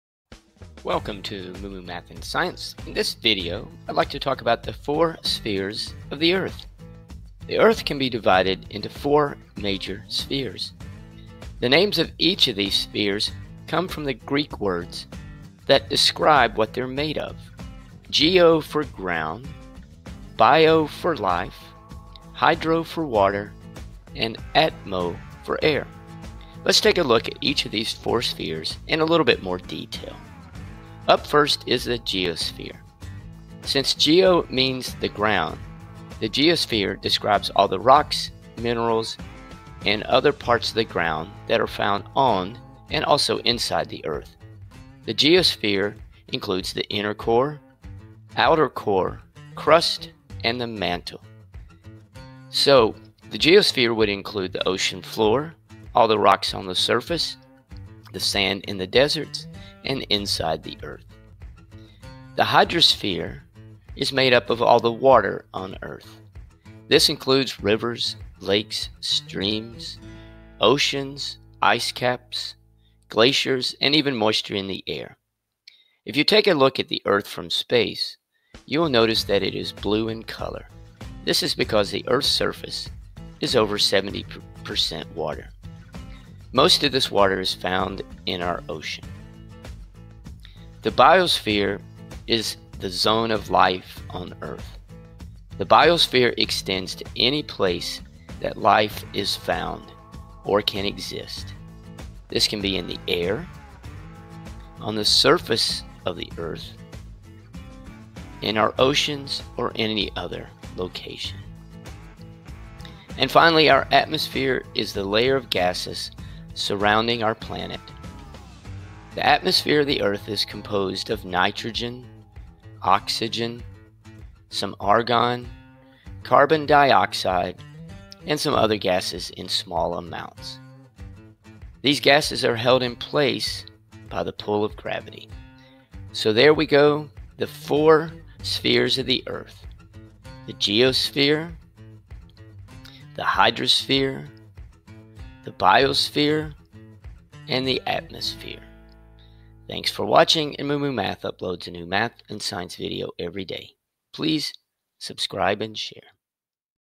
4 spheres of the earth
Learn about the four Spheres of the earth.
The earth can be divided into four major spheres.
The biosphere which means life.
The hydrosphere which makes up all the water of the earth
The atmosphere which is the air of the earth
The geosphere which is all of the rocks, soil, and layers of the earth.

These four spheres of the earth allow life to exist on planet earth